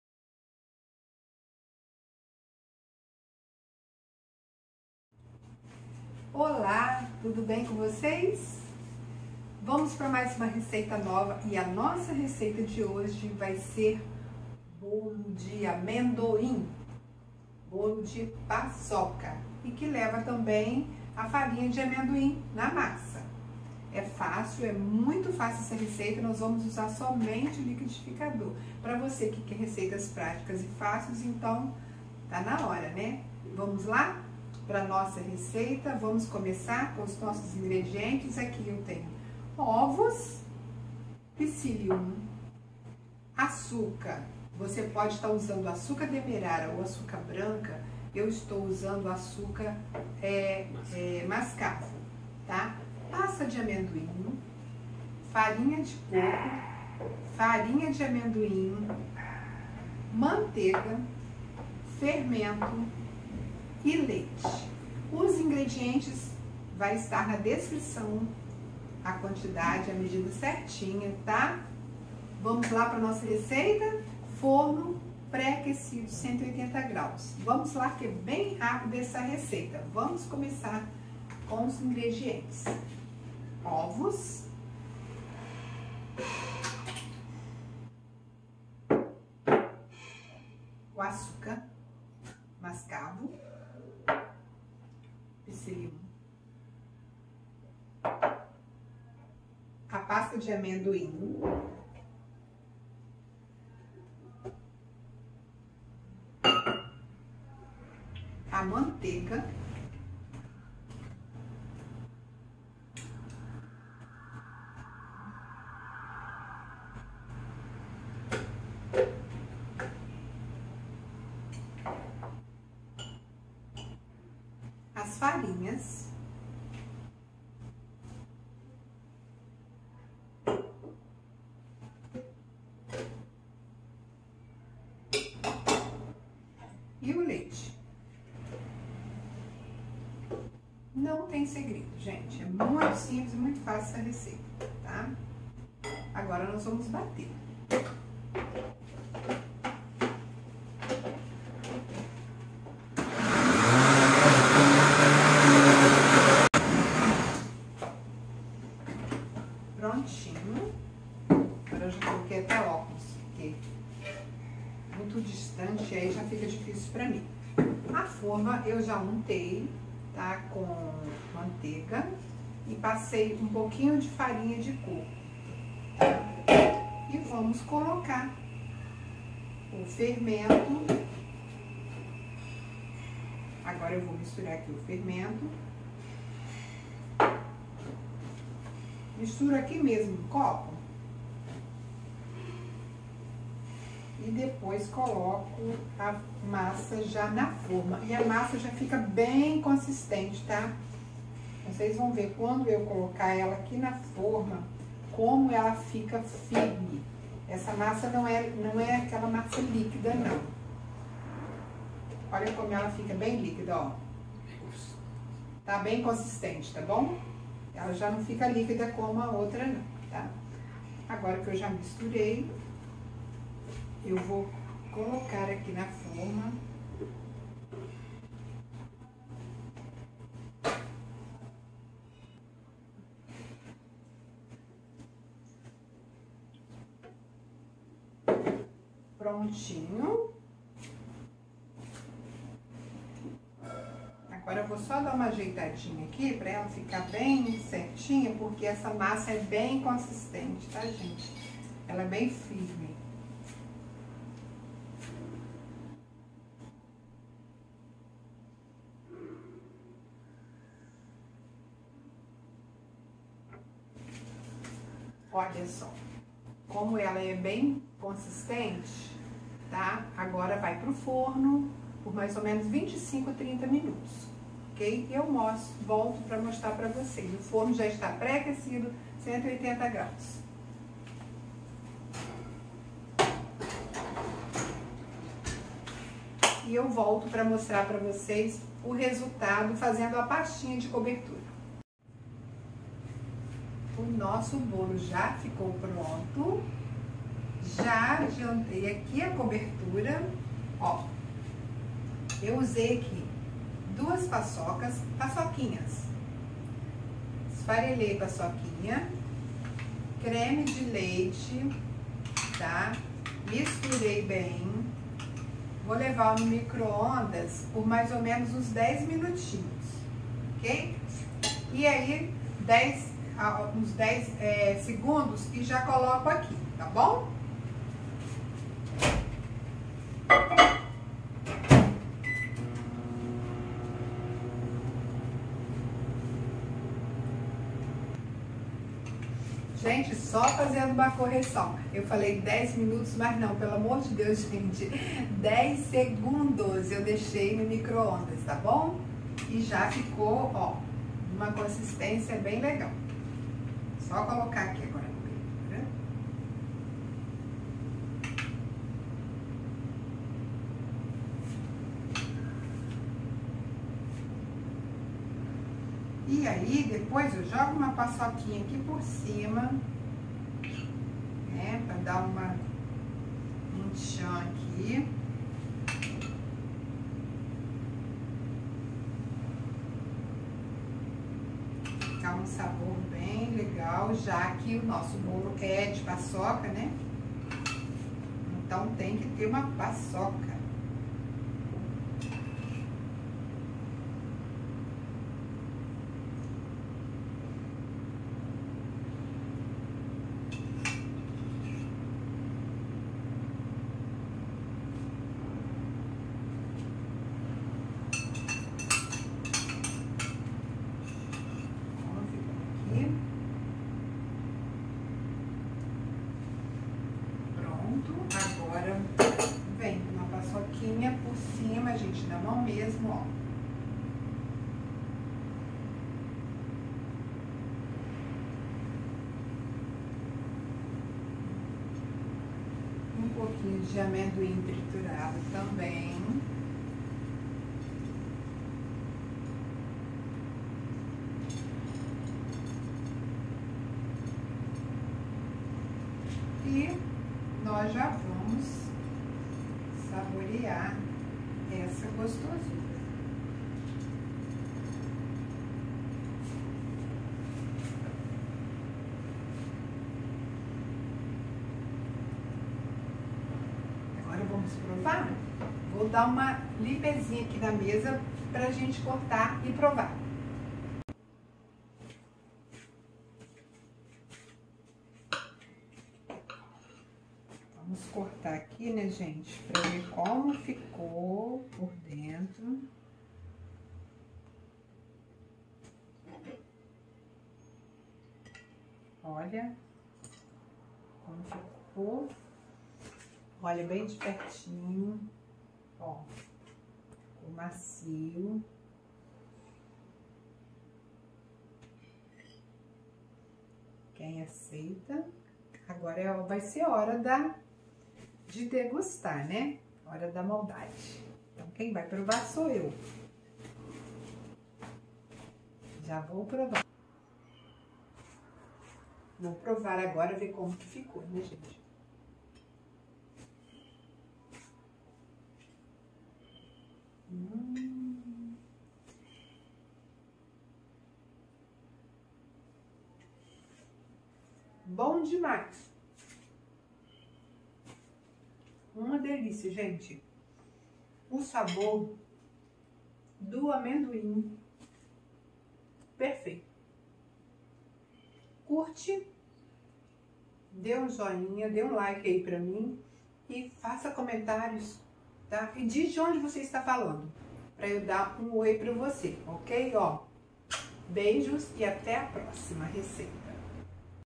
BOLO PAÇOCA de liquidificador.
Hoje trouxe esta receita maravilhosa de Bolo Paçoca de liquidificador, saudável e  que dá água na boca.
Esta receita  é para facilitar dando praticidade no preparo com um sabor maravilhoso e leve. Aqui em casa amamos esta receita, afinal de contas amendoim é muito bom. E confesso que esse bolo acabou muito rápido. Rs

Obrigada por todo carinho!

A RECEITA AQUI 👇

- 03 ovos
- 03 colheres de sopa de açúcar demerara
- 02 colheres de sopa de de manteiga
- 200 ml de leite desnatado
- 01 xícara de farinha de amendoim
- 1/2 xícara de farinha de Coco
- 03 colheres de sopa de psilium
- 01 colheres de sopa de fermento em pó.

COBERTURA
- 1/2 cx de creme de leite
- 03 paçocas esfareladas
- 01 colher de pasta natural de amendoim
- 02 paçocas para fazer o acabamento.

Seja feliz!
Bjos da Ester 😘